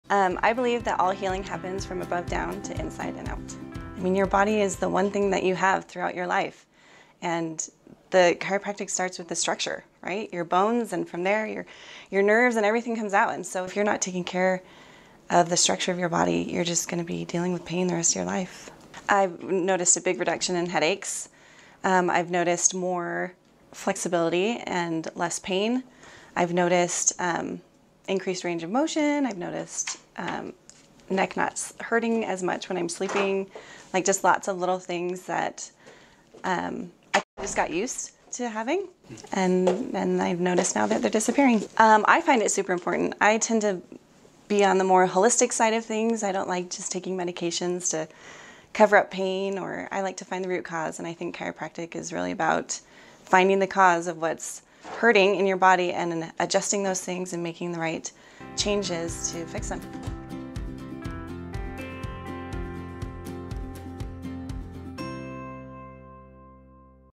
Christine F. How healing works above down inside out
You want success in healing?  Here is how it works!  Christine explains how she prefers a more holistic approach to better health for her and her family.